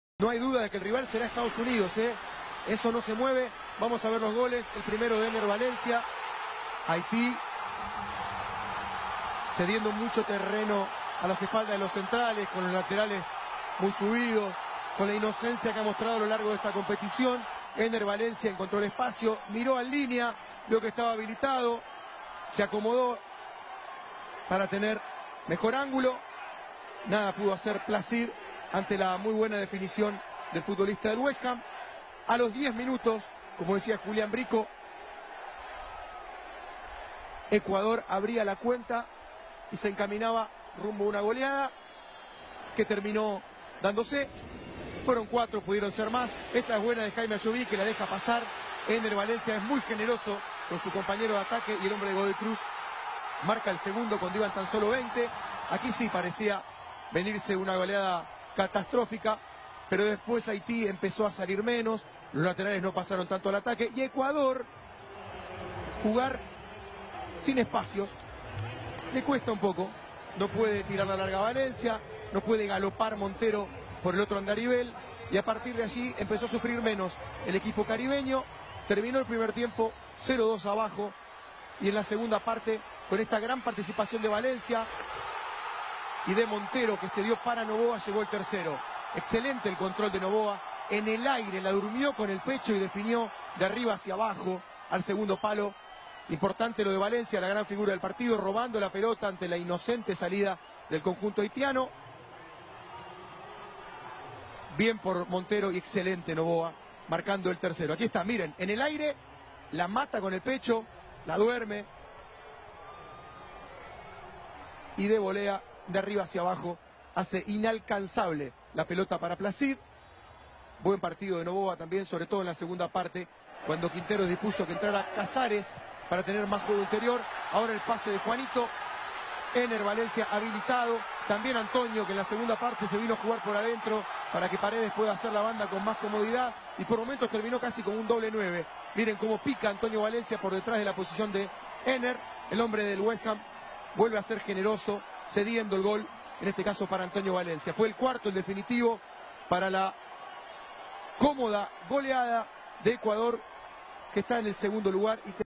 Ecuador 4-0 Haiti copa america centenario-tyc sports
Goleada y enfrentara a USA, para mi ,pasa USA